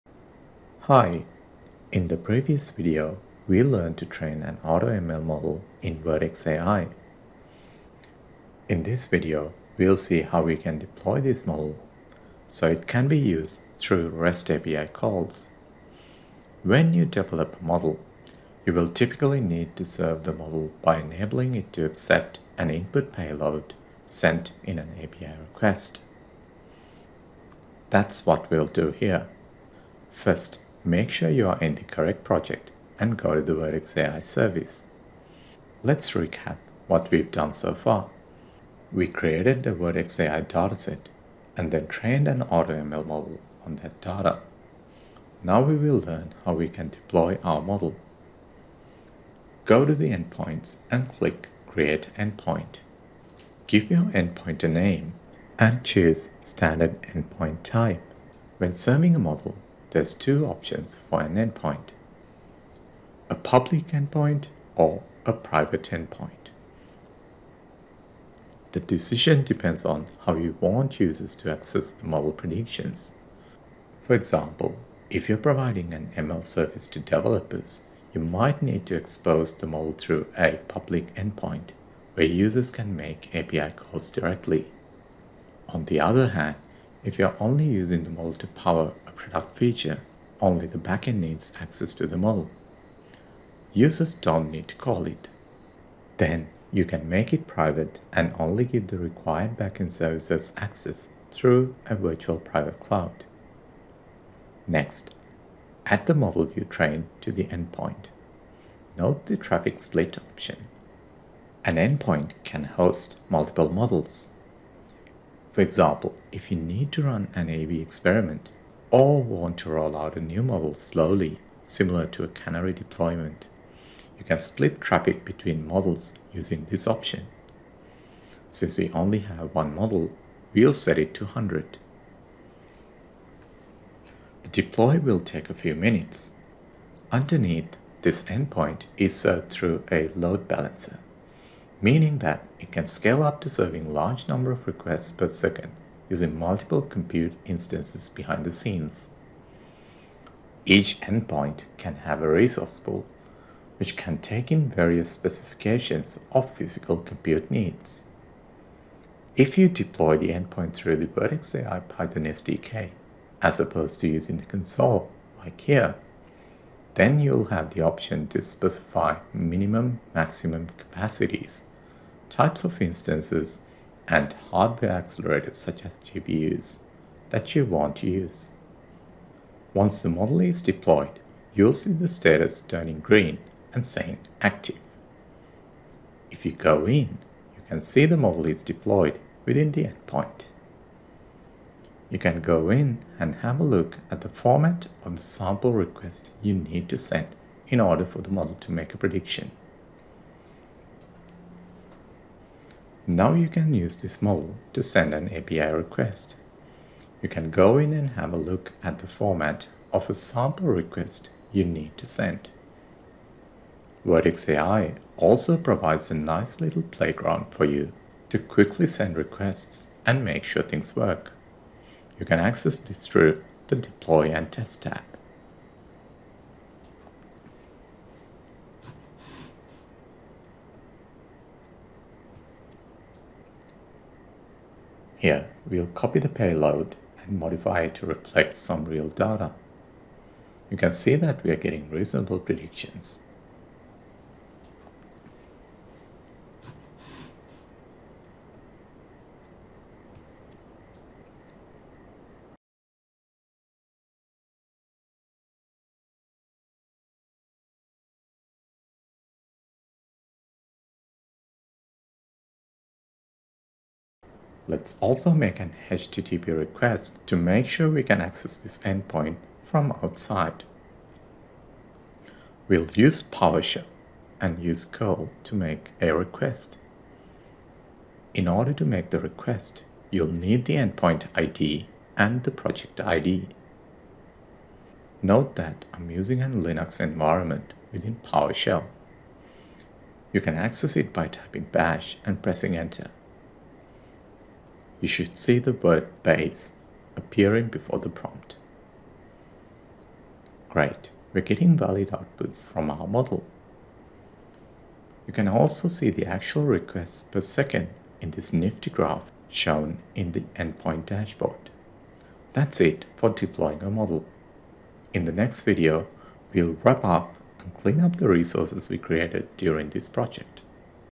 ML with Vertex AI - Text classification (Part 4 - Deploying a model)
In this video, we will be deploying our trained model. This is the fourth video of a video series that teaches to do machine learning on Vertex AI

Chapters
0:00 Introduction
0:35 Recap
0:46 Creating the endpoint
1:44 Deploying the model
3:15 Sample request
3:38 Testing the model
4:25 Making an HTTP request
5:11 Wrapping up